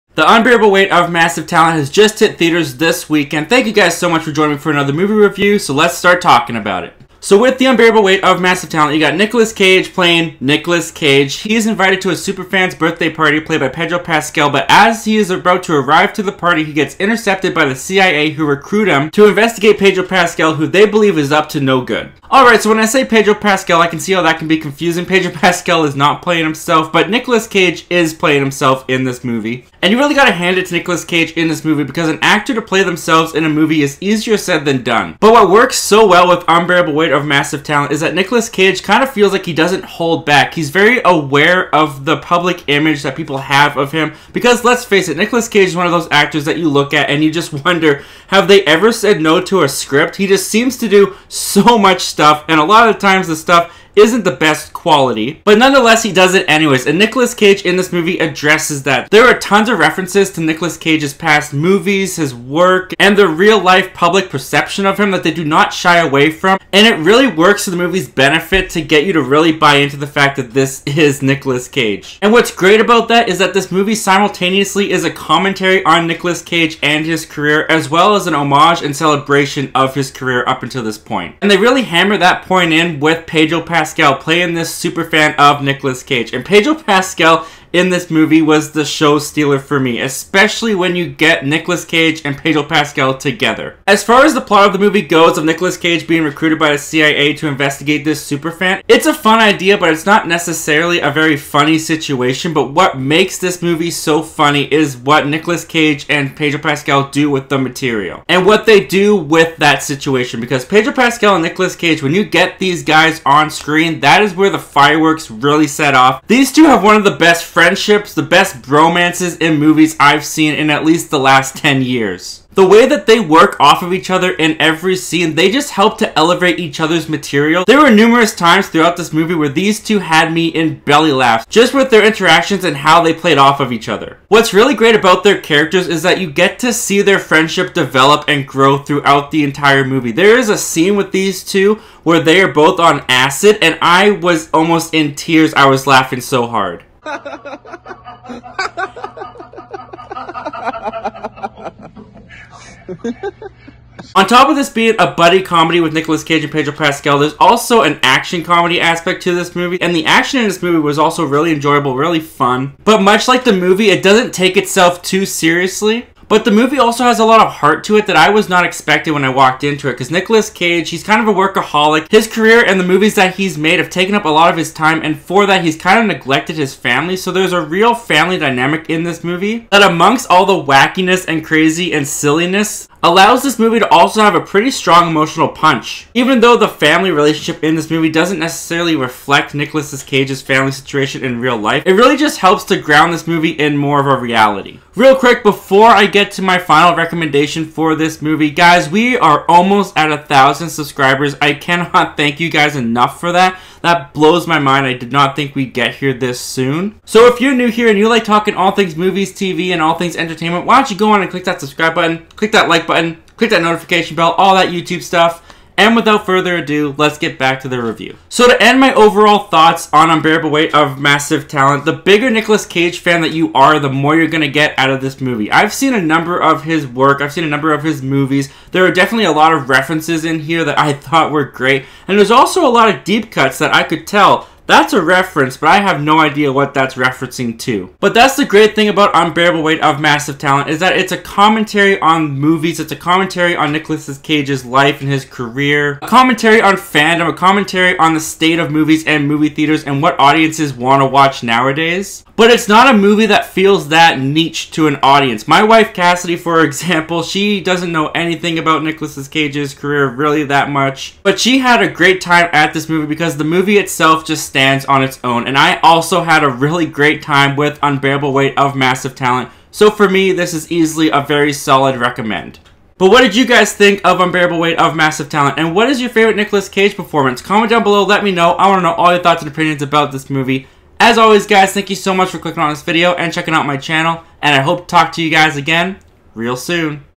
The Unbearable Weight of Massive Talent - Movie Review
Nicolas Cage is back in theatres this weekend with “The Unbearable Weight of Massive Talent.” Here are my thoughts on the new self aware comedy, starring Nicolas Cage as Nicolas Cage!